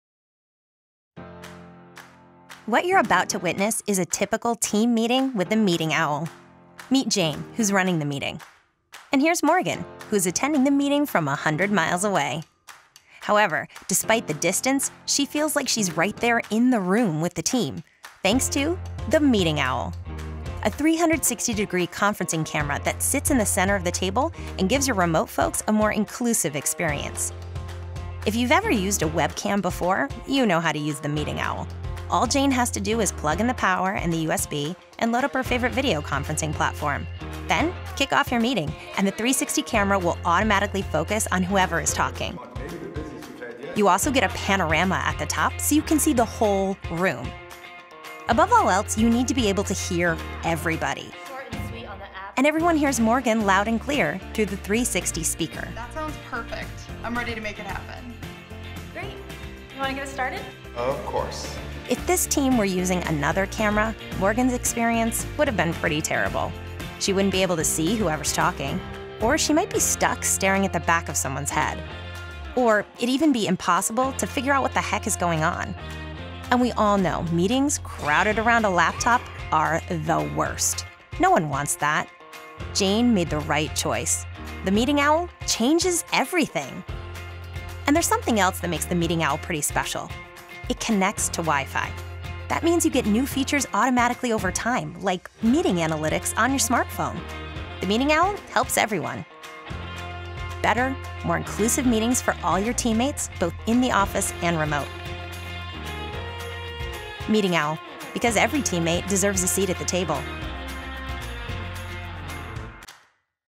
The Meeting Owl
No other conferencing camera puts everyone “in the room.” Take a transformational approach to your remote and distributed meetings. The Meeting Owl gives a more inclusive video conferencing experience. That means increased efficiency, no miscommunication, and a team that feels closer together.

BEST TEAM MEETING EXPERIENCE. Meeting Owl is the first 360° smart video conferencing camera. It automatically highlights and shifts focus to different people in the room when they speak, so you know what’s happening as if you’re there.

SEE AND HEAR EVERYONE CLEARLY. It combines mic, camera and speaker into one device and sits in the center of the table. The 360° lens shows the whole room, while eight microphones pick up everyone within twelve feet.

EASY AND QUICK SETUP. Plug in the power and USB, load up your favorite video conferencing platform, and start your meeting. No downloads or installs.

GET NEW FEATURES AUTOMATICALLY. Meeting Owl connects to wifi so you get new helpful features automatically over time and the best possible experience.

GIVE THE OWL A TRY. Use our 30-day return policy to try the Meeting Owl in your own office. Get feedback from your in-office and remote teammates and see what they think.